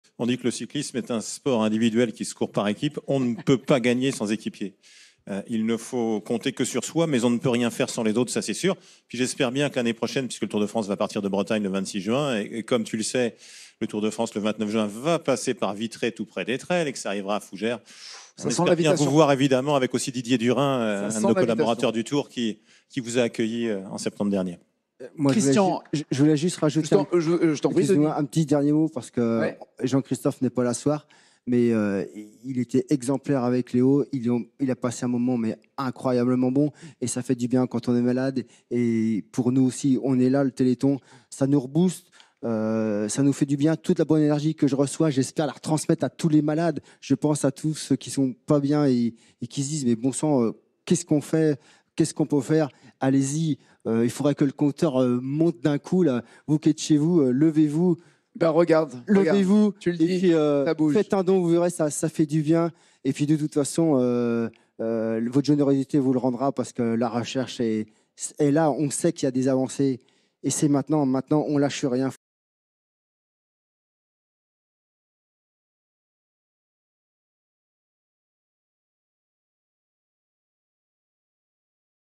Denis, le papa de Léo, remercie Jean-Christophe Peraud et le Téléthon
Denis, le papa de Léo, tient à remercier Jean-Christophe Peraud qui a fait avec Léo le défi en tandem. Mais également le Téléthon qui fait du bien aux familles, les rebooste. "On sait qu'il y a des avancées dans la recherche, et c'est maintenant, on ne lâche rien."

Le Téléthon, c’est maintenant.